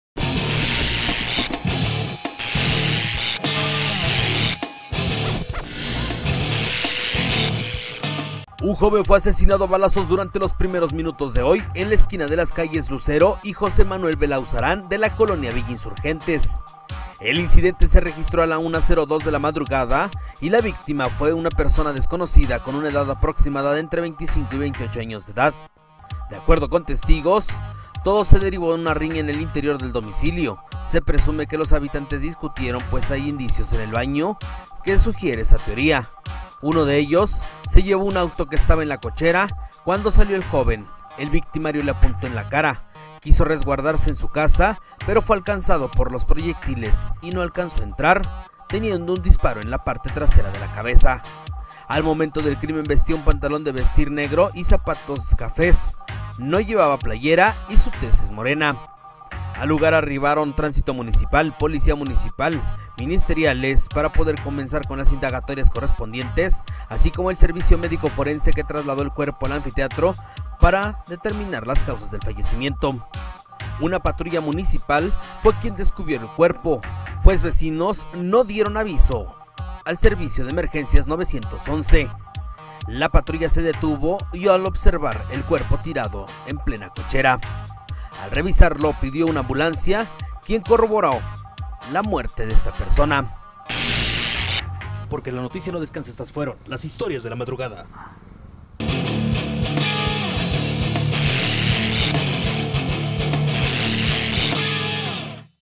Historias de la Madrugada 9 de Noviembre 2016
A continuación te presentamos los sucesos policiacos ocurridos durante la madrugada